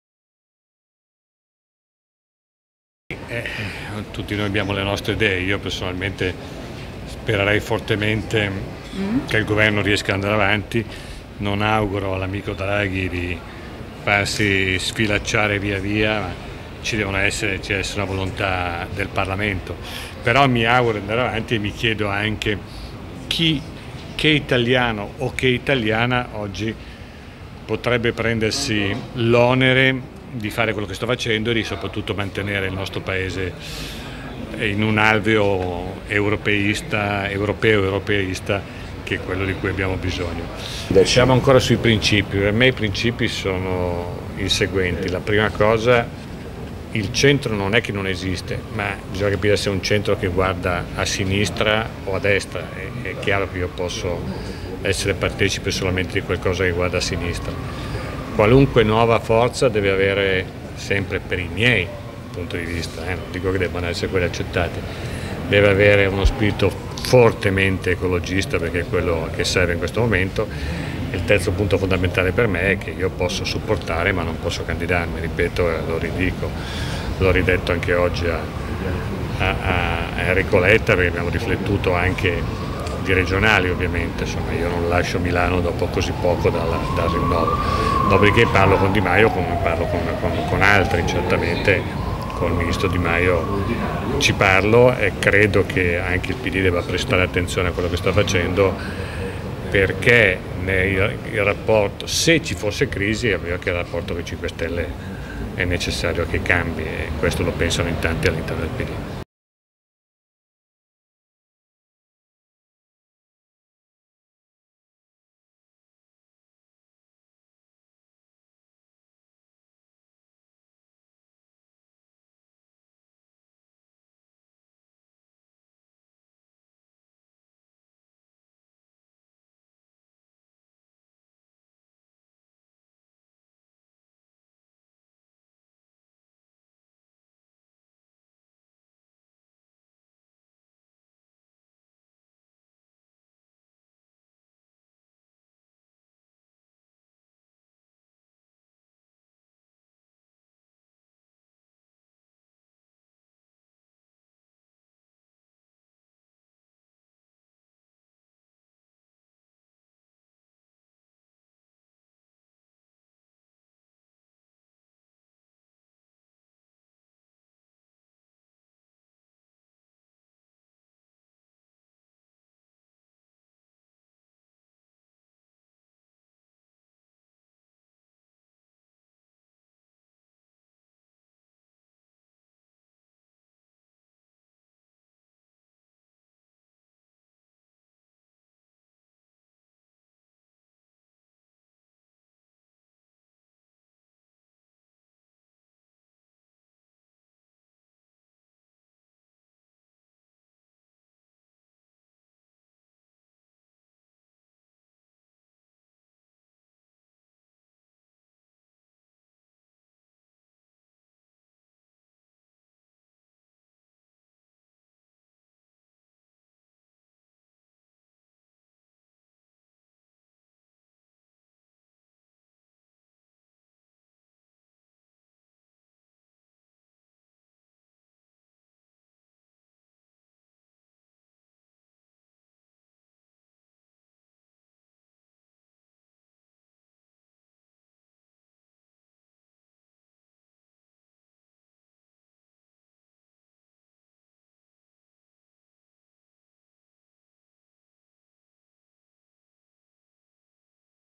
GOVERNO, SALA: SPERO FORTEMENTE VADA AVANTI, CHI PRENDEREBBE ONERE DRAGHI?
(MIANEWS) Milano, 14 LUG - “Io spererei fortemente che il governo riesca ad andare avanti. Ci deve essere una volontà del Parlamento. Mi chiedo anche che italiano o italiana oggi potrebbe prendersi l’onere di quello che sta facendo Draghi e di mantenere il nostro Paese in un alveo europeista”. Lo ha detto il sindaco Giuseppe Sala, a margine delle celebrazioni per l'anniversario della Fondazione Cariplo al Piccolo Teatro Strehler, a chi gli domanda com’è andato l’incontro con il segretario del PD Enrico Letta. “Abbiamo ovviamente parlato delle questioni politiche per cercare di capire quali sono le evoluzioni possibili”, ha precisato Sala.